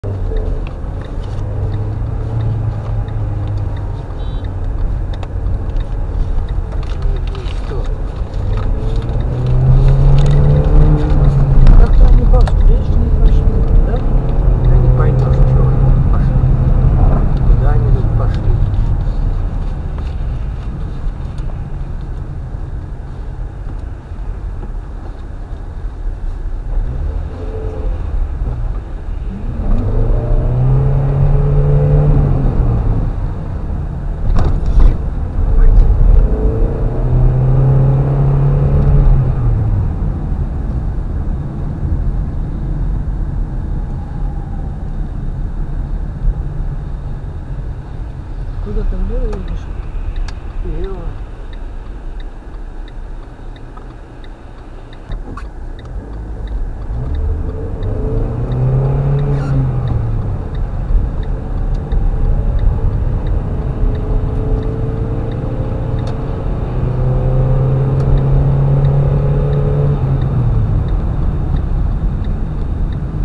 С улицы 12 Лет Октября на Кашена, Колхозную площадь
Проезд перекрестков. Смоленские улицы. Как проехать. Смоленск.